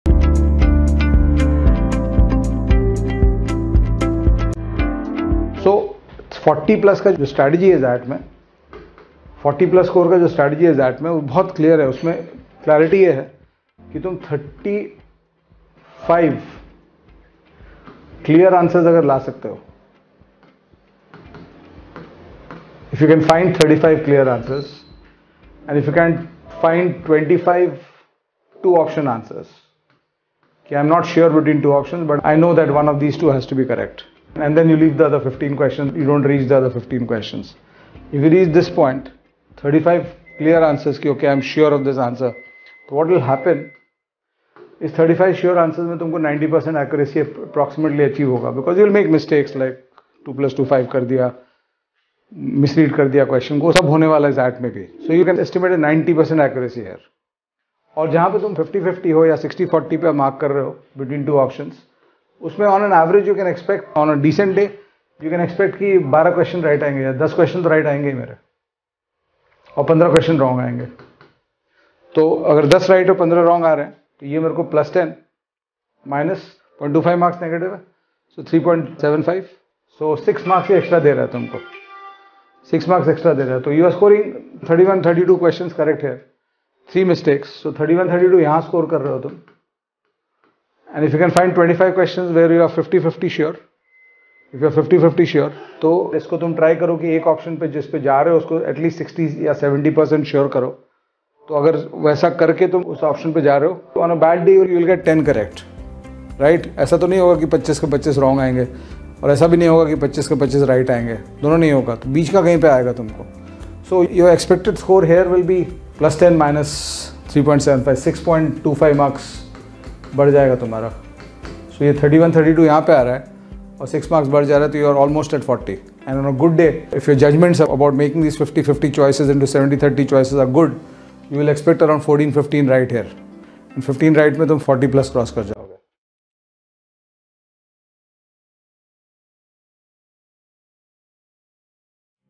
The Strategy To Reach 40+ Marks In XAT For 99 Percentiles | XAT Preparation | XAT Quants | XAT DI
In this video, Arun Sharma Sir is giving you some strategic tips about XAT Quants & DI questions to boost your XAT Preparation. It will also give you ideas about how to crack XAT 2023.